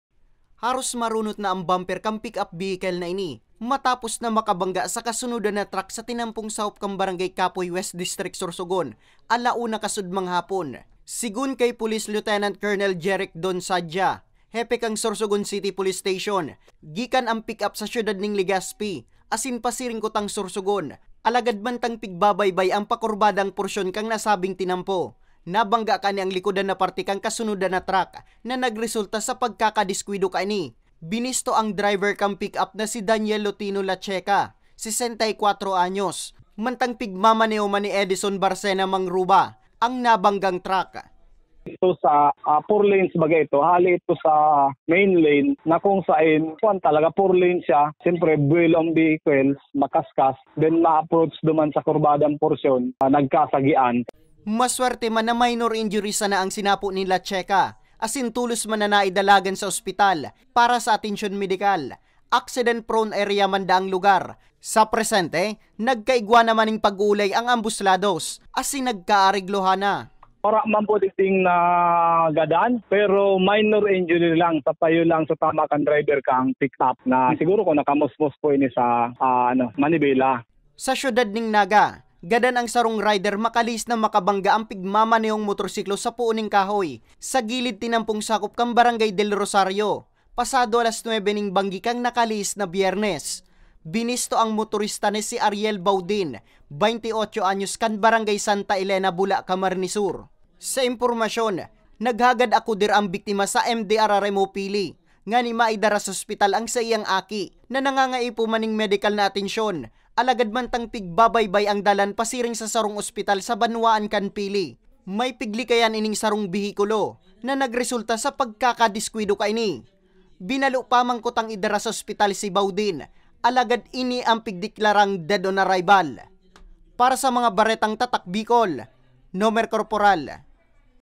Saro, gadan, saro, irido sa duwang mag kasuhay na diskwido sa Camarines Sur asin Sorsogon
Saro, gadan, saro, irido sa duwang mag kasuhay na diskwido sa Camarines Sur asin Sorsogon | via Nomer Corporal